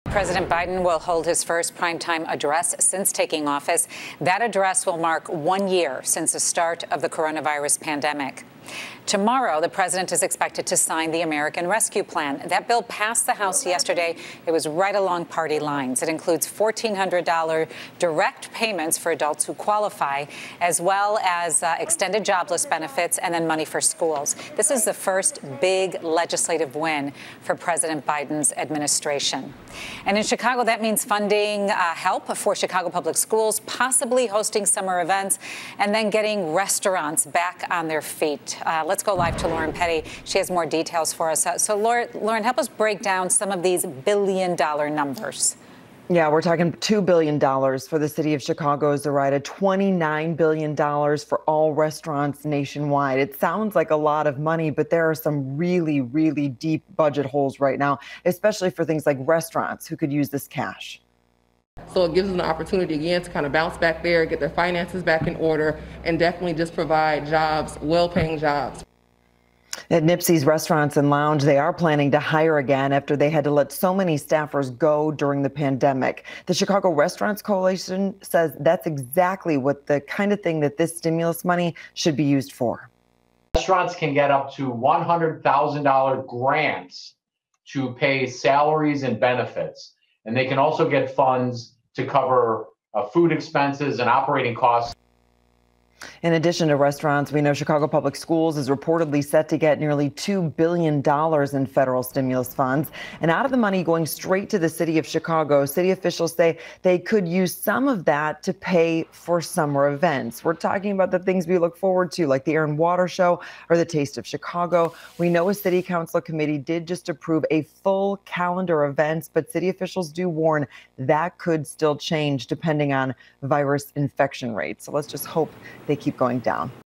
What the COVID Relief Bill Means for Chicago and Illinois | NBC Chicago
What impact with the $1.9 trillion COVID relief bill that passed Congress on Wednesday have on Chicago and the state of Illinois? Lauren Petty has details.